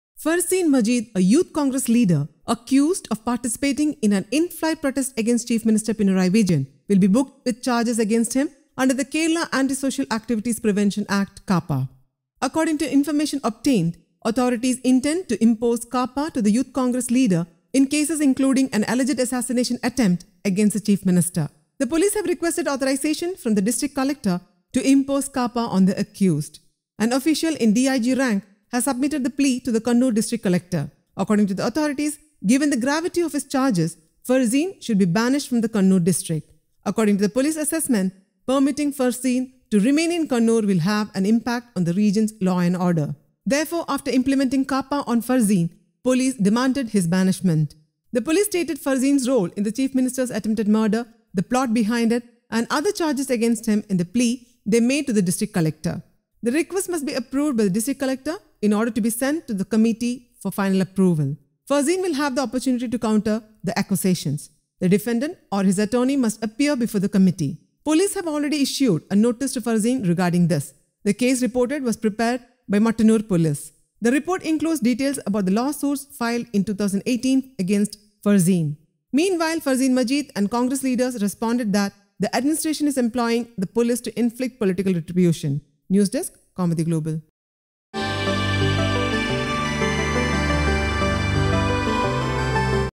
Youth Congress leader accused in the In-flight protest will be booked under KAAPA
Farseen Majeed, a Youth Congress leader, accused of participating in an in-flight protest against Chief Minister Pinarayi Vijayan will be booked with charges against him under the Kerala Anti-Social Activities Prevention Act (KAAPA). According to information obtained, authorities intend to impose KAAPA to the Youth Congress leader in cases including an alleged assassination attempt against the Chief Minister.